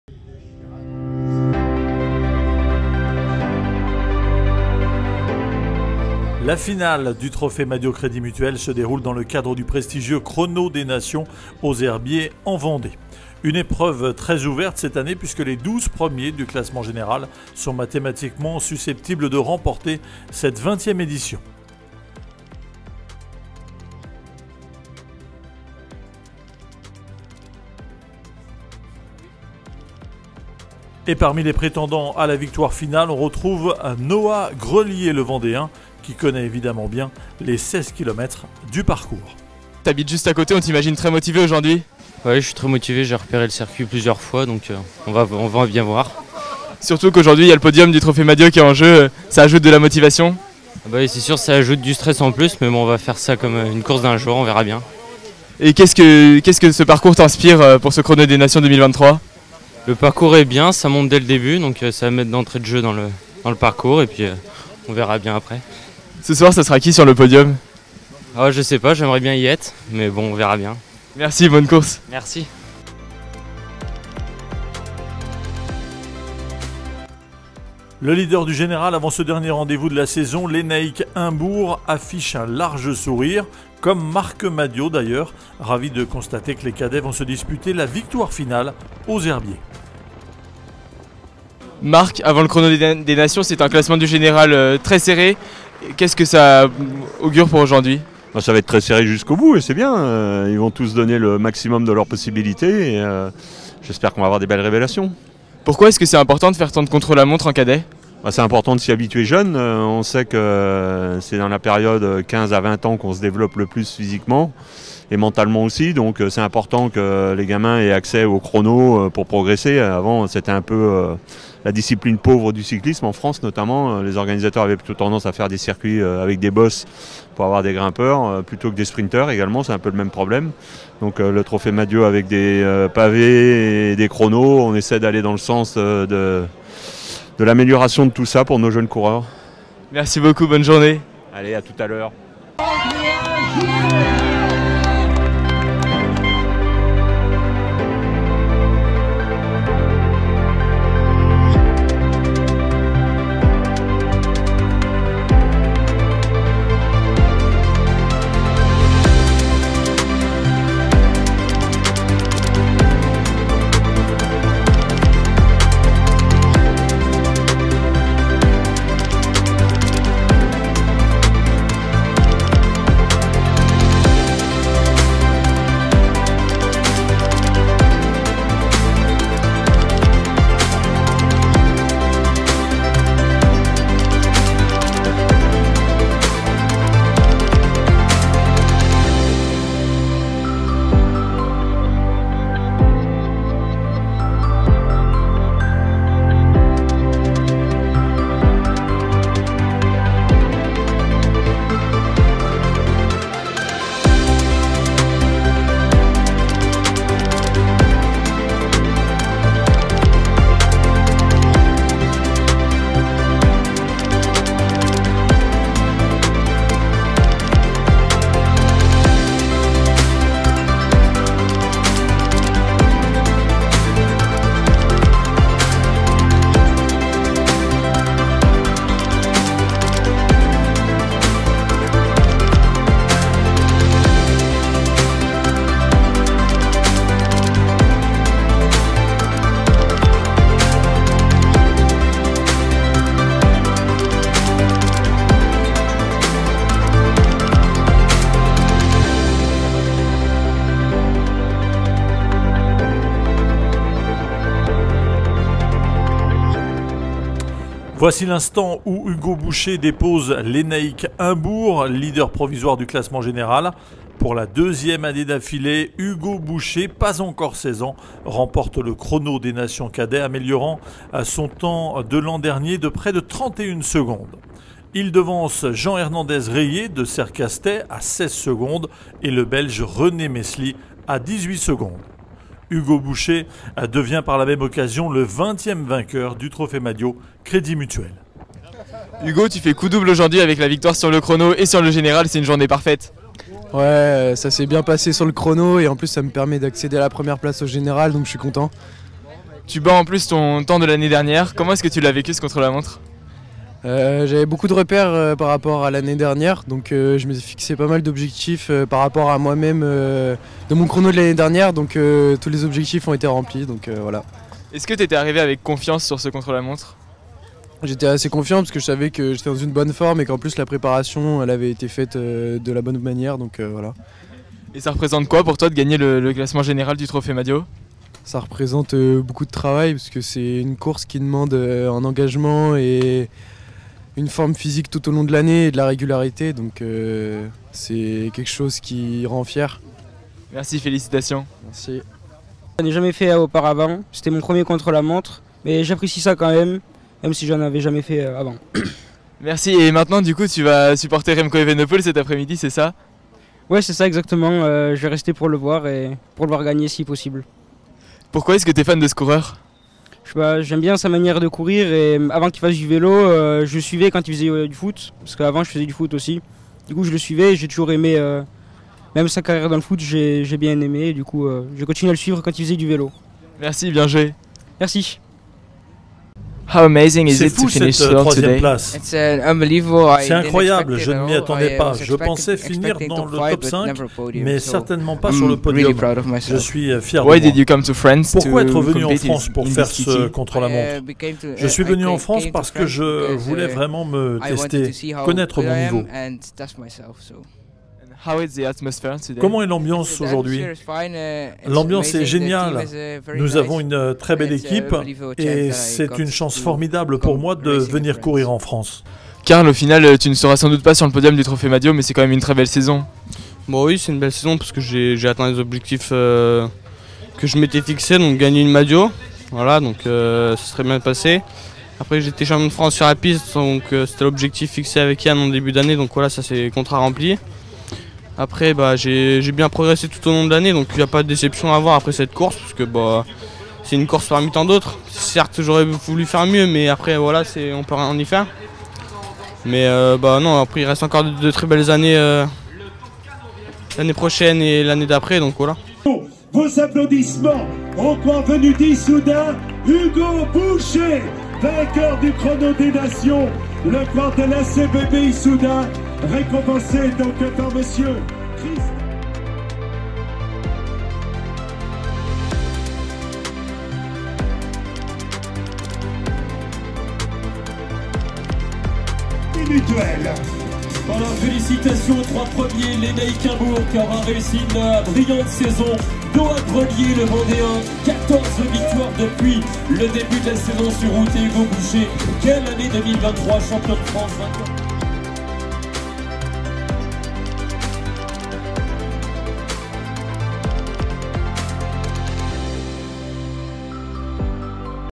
Trophée Madiot 2023: Chrono des Nations/finale
Vidéo reportage de Pascal Normand aux Herbiers (Vendée): finale du Trophée Madiot-Crédit Mutuel 2023. Avec les interviews de Tristan Dereeper-Bidet.